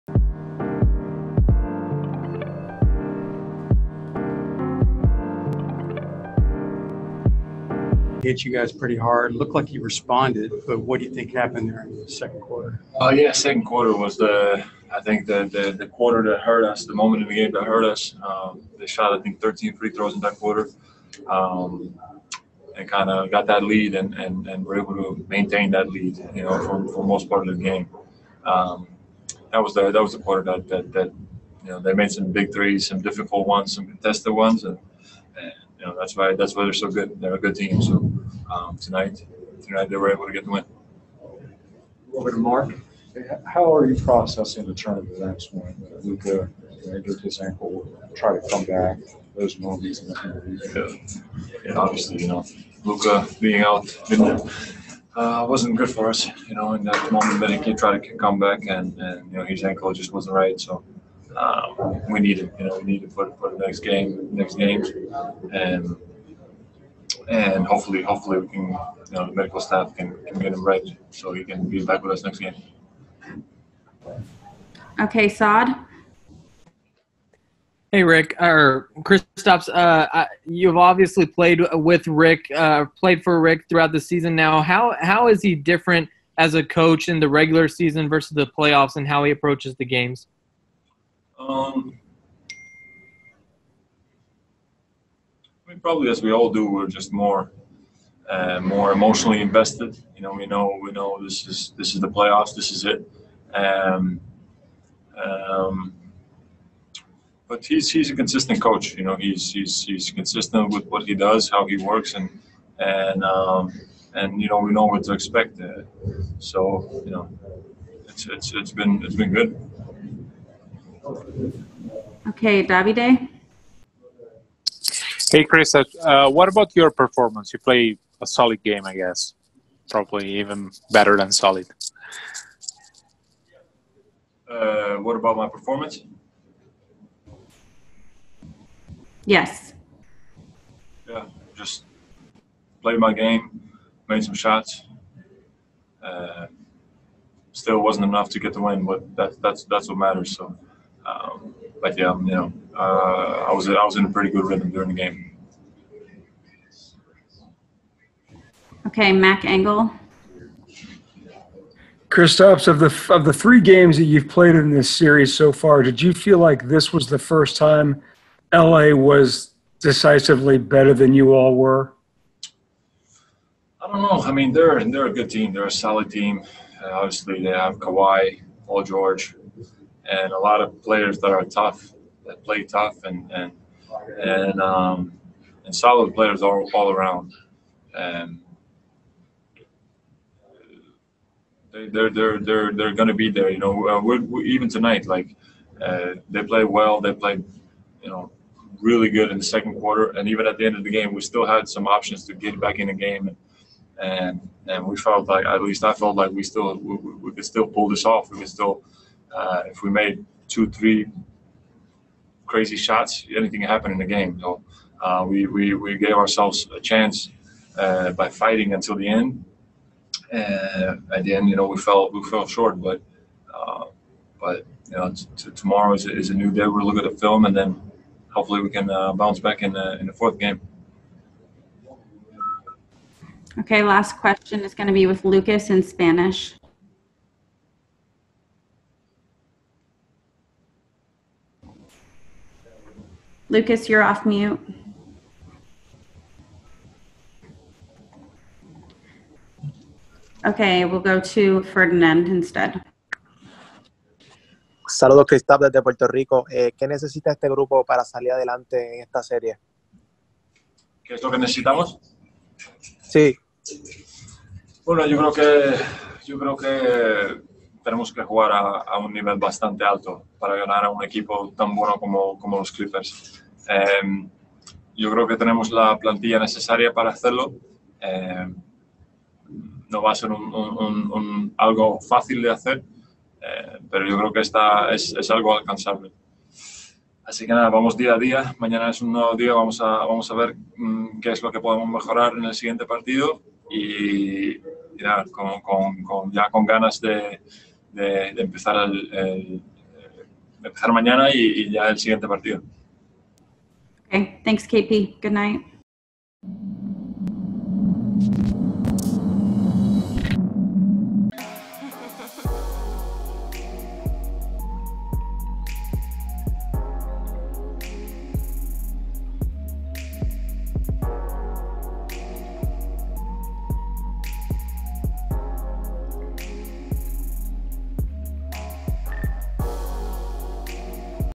Kristaps Porzingis Reacts to Luka Doncic Ankle Injury & Answers if LA Clippers Are Better Than Mavs
Watch as Kristaps Porzingis Reacts to Luka Doncic Ankle Injury & Answers if LA Clippers Are Better Than Mavs After Game 3 (CLIPPERS vs. MAVS GAME 3, POST-GAME)